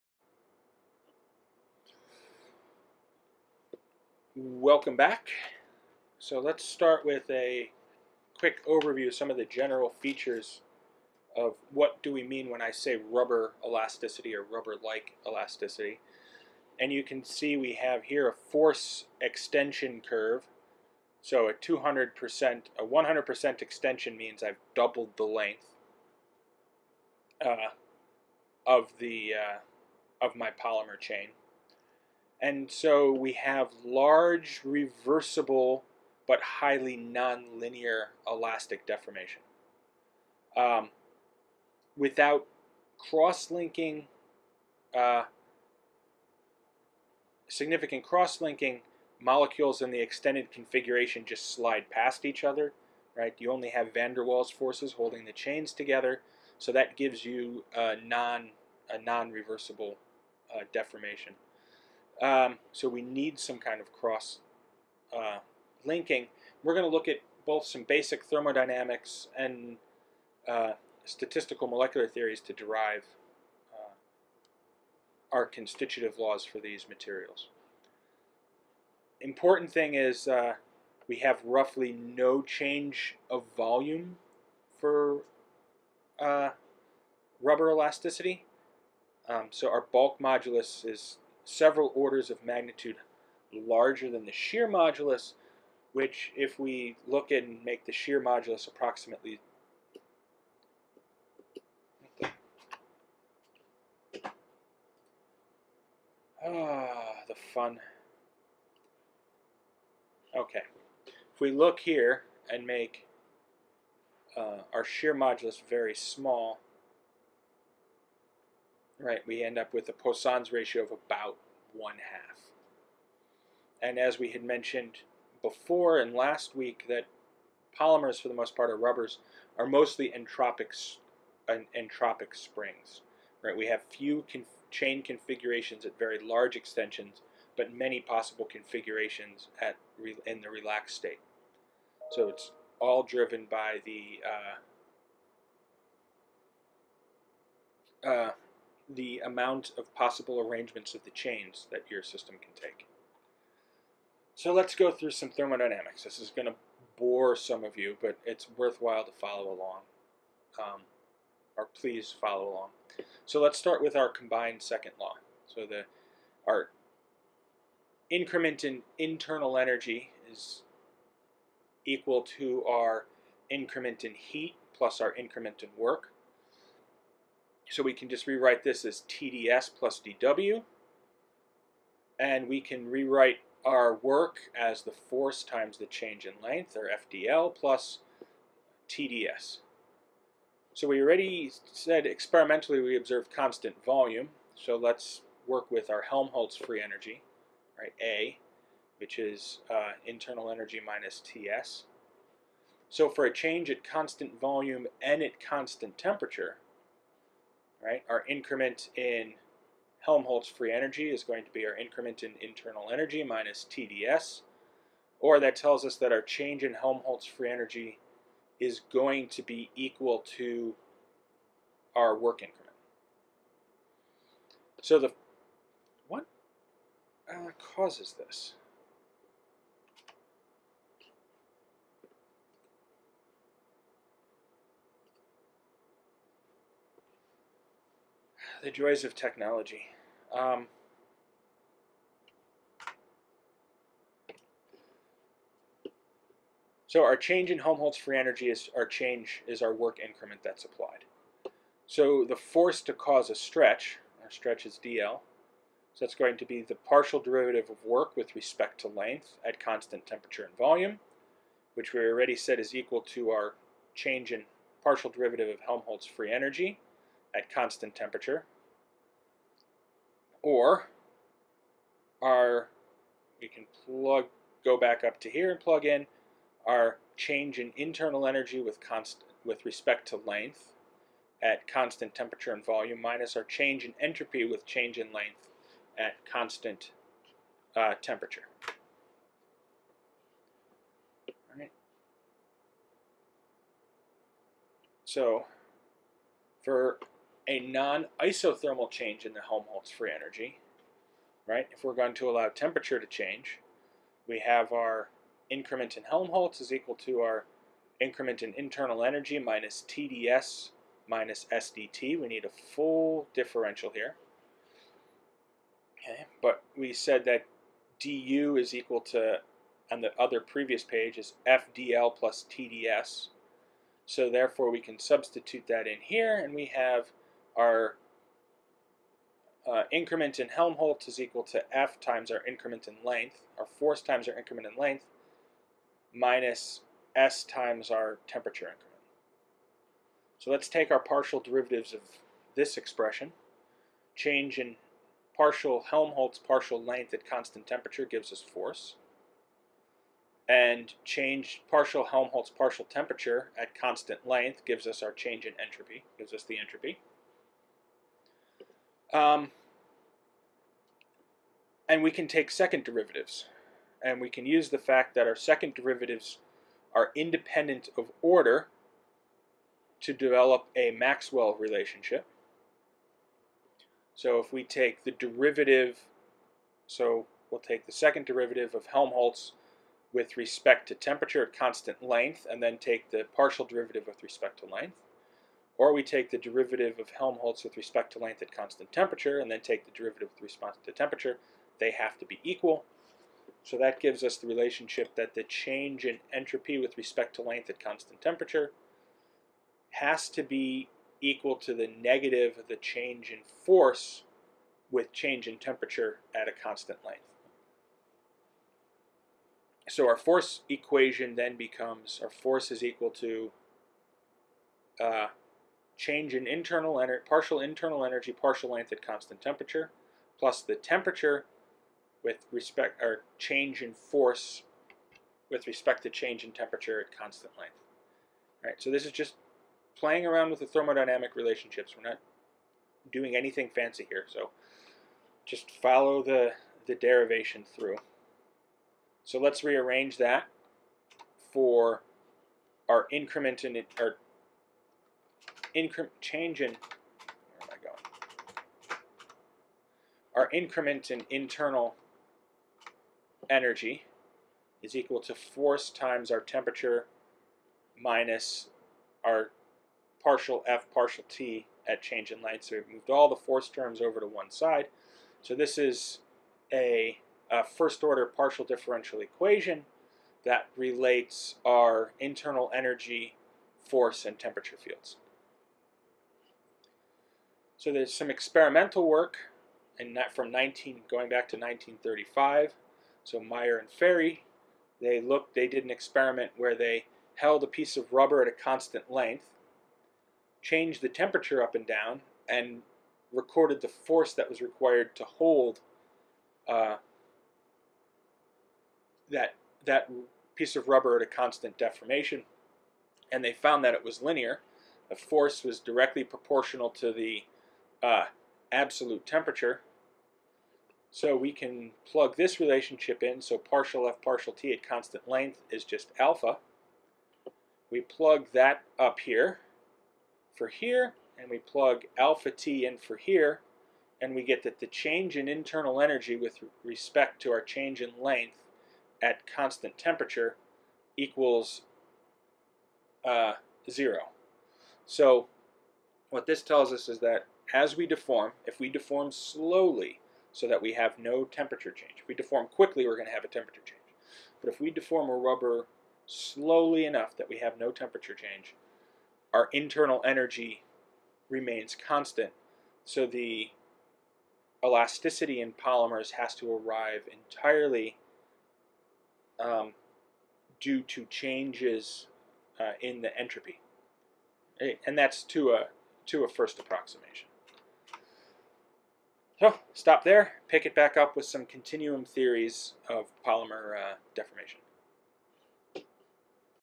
Lecture 4 Part 2 Thermodynamics of Polymer Elasticity
Brief derivation of the entropy effects of polymer elasticity